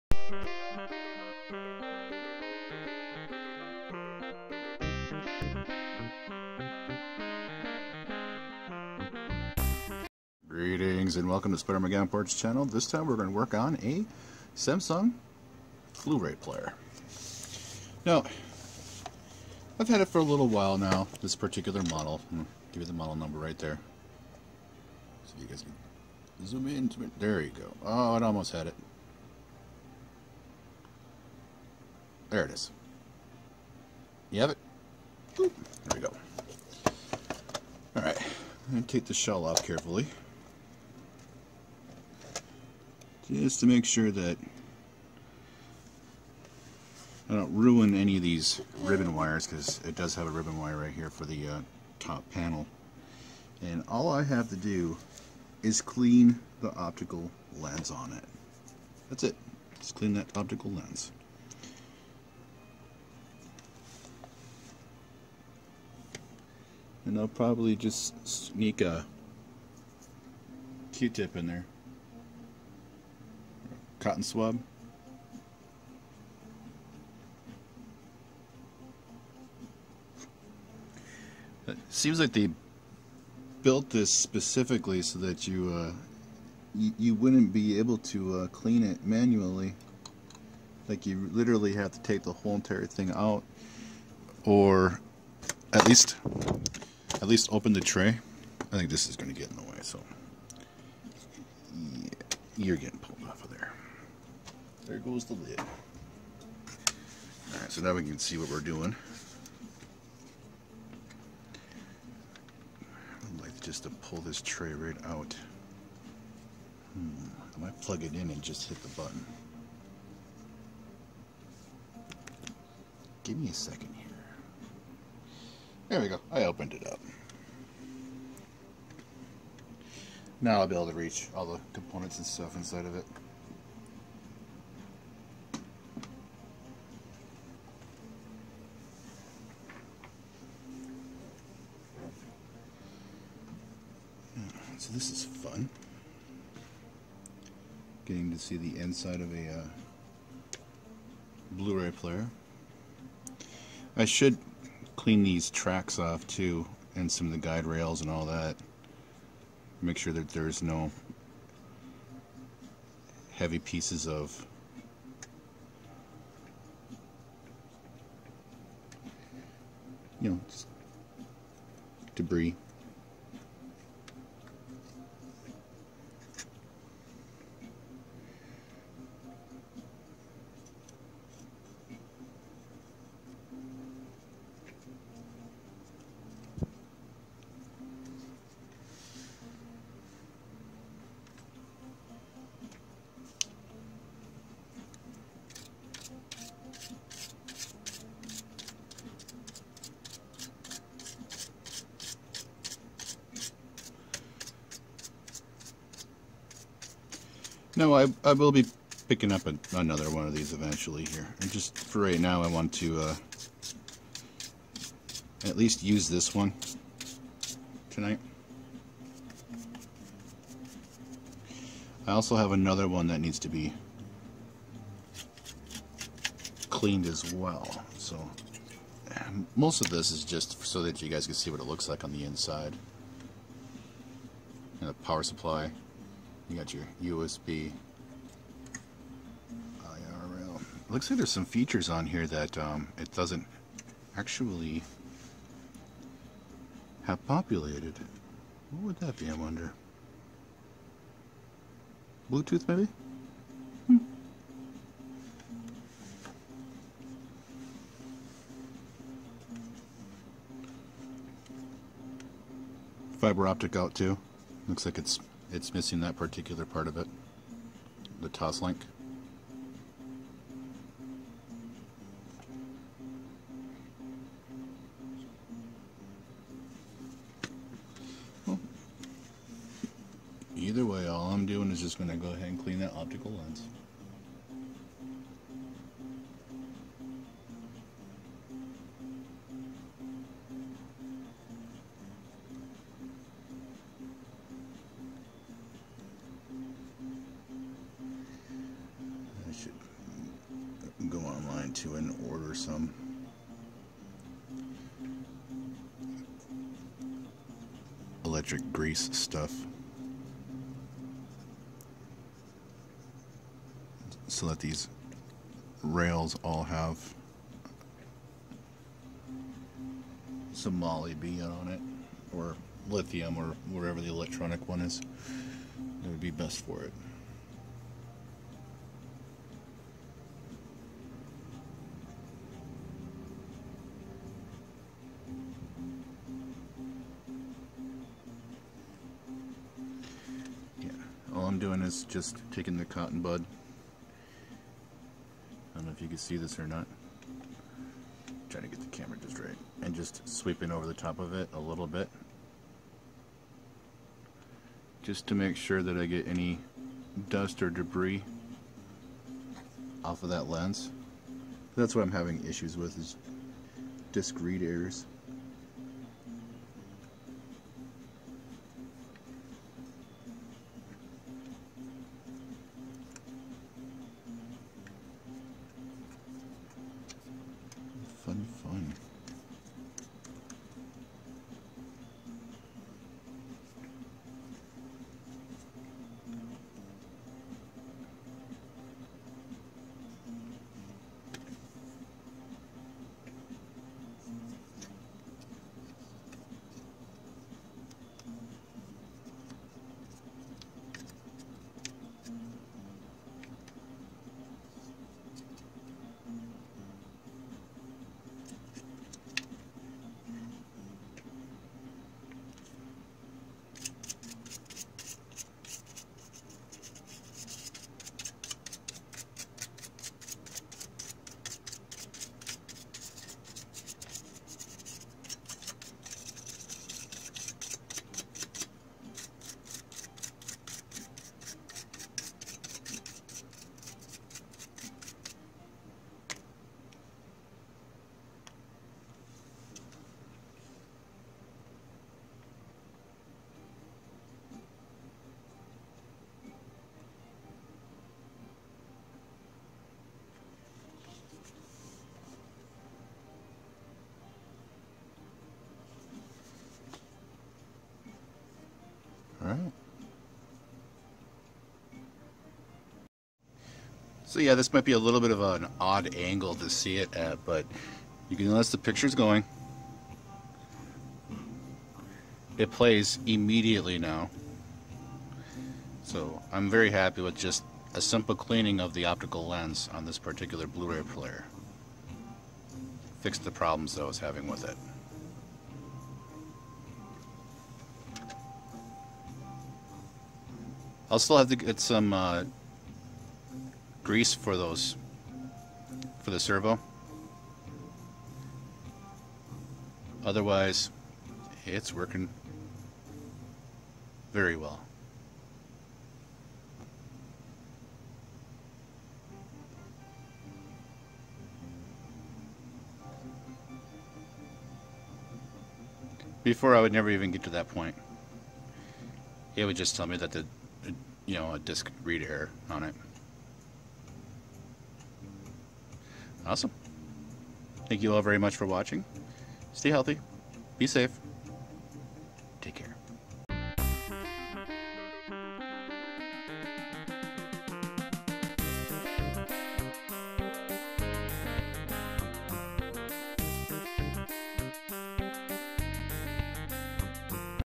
Samsung Blu-ray Player | Cleaning
Samsung Blu-ray Disc Player Model: BD-HM51 | Cleaning

Well my Samsung Blu-ray Player was giving me read disc errors, and I decided to take it apart and clean its optical lens which worked flawlessly. This was fun and interesting to see the inside of a blu-ray player not my usual disassembly cleaning videos. But was lots of fun and easy to take apart. Its a wonderful idea to open the tray after the top cover is off however be very careful doing so as the powered equipment without a cover can be dangerous. So please use all precautions. Safety is paramount.

Share Like Emulate